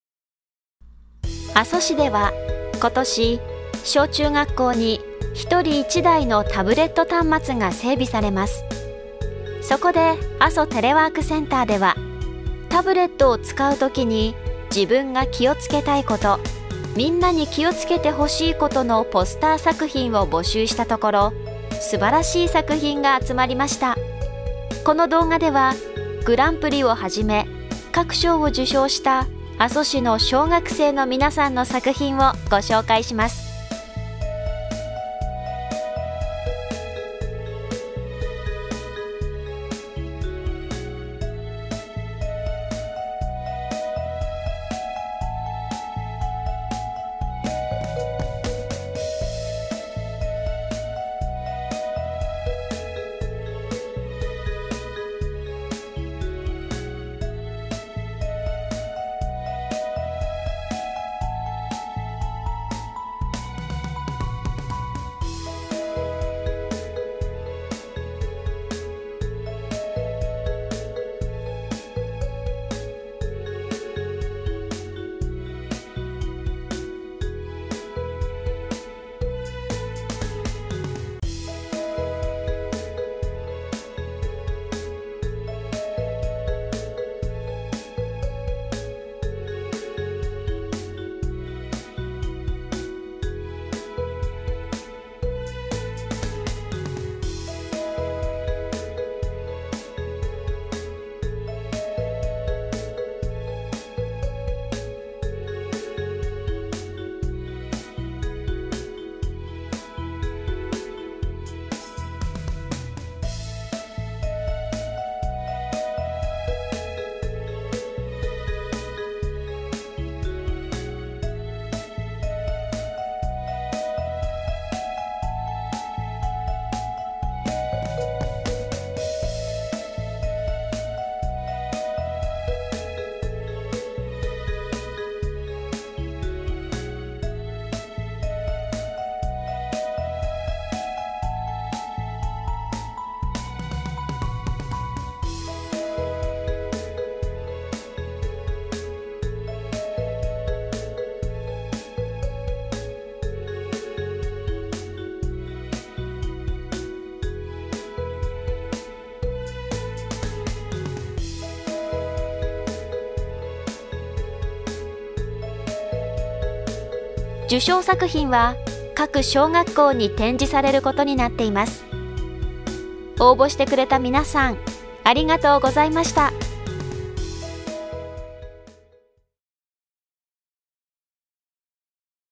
情報モラル啓発ポスター受賞作品紹介
阿蘇市では今年小中学校に1人1台のタブレット端末が整備されます。そこで阿蘇テレワークセンターでは、タブレットを使うときに自分が気を付けたいこと、みんなに気を付けてほしいことをポスターにして応募を募ったところ、素晴らしい作品が集まりました。

この動画では、グランプリをはじめ各賞を受賞した阿蘇市の小学生の皆さんの作品をご紹介します。